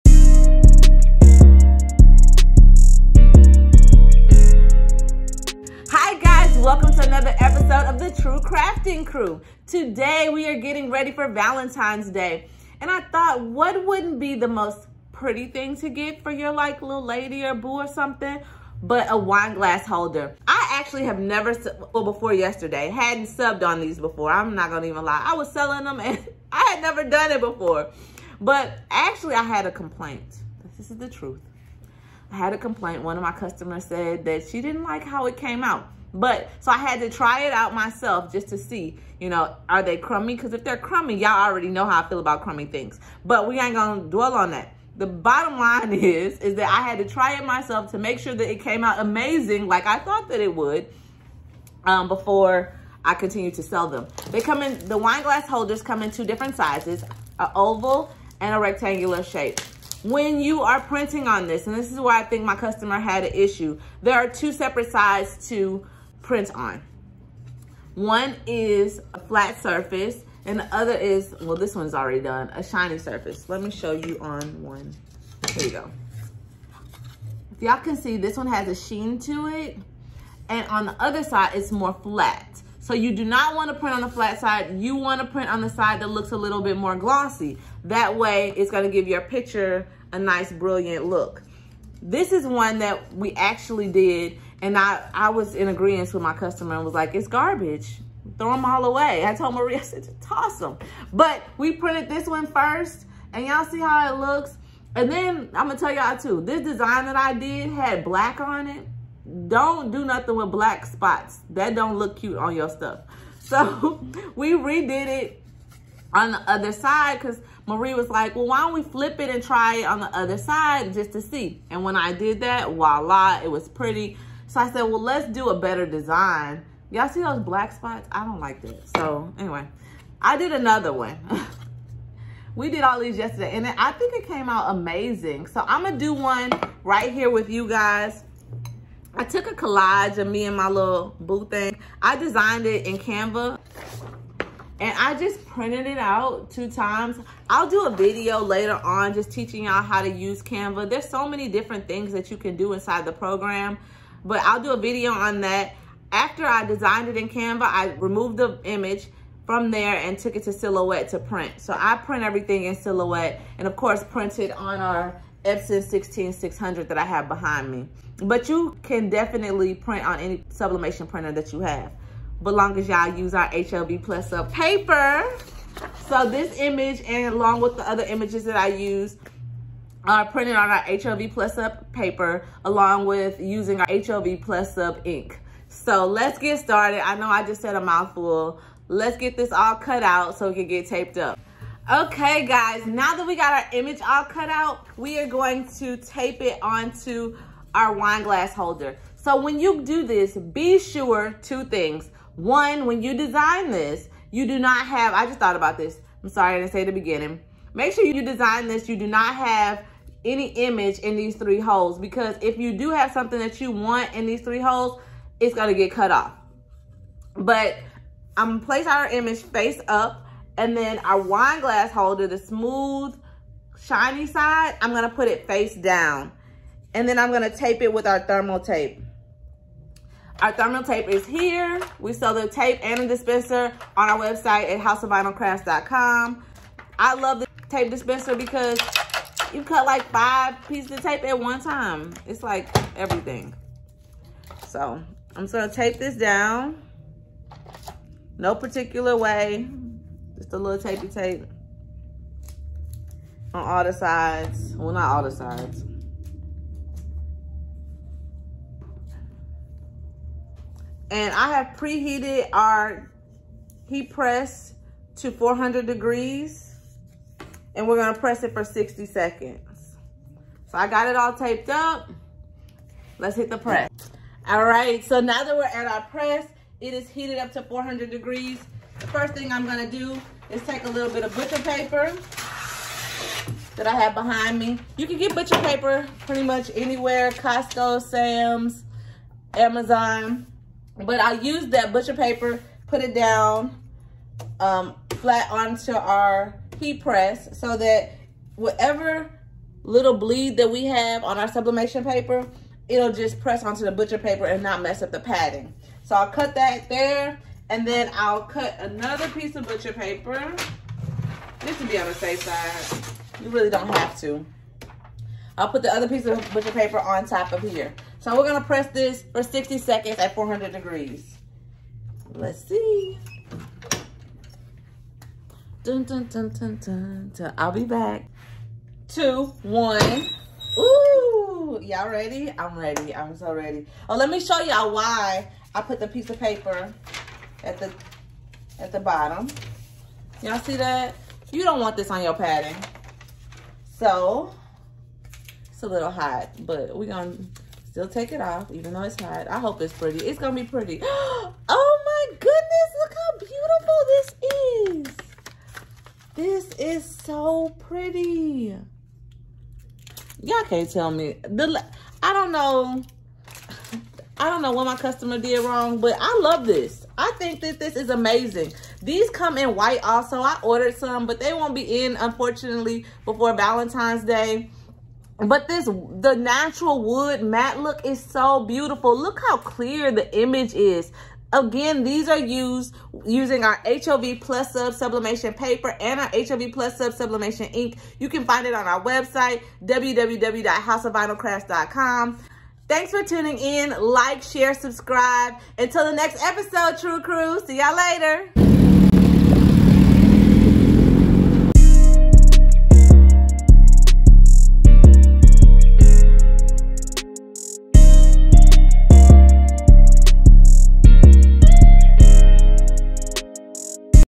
How to Sublimate a Wine Glass Holder
In this video I will be showing you how sublimate on a wine glass holder. They are absolutely amazing. You should definitely try one if you haven't already. All products in this video can be purchased

Butcher Paper: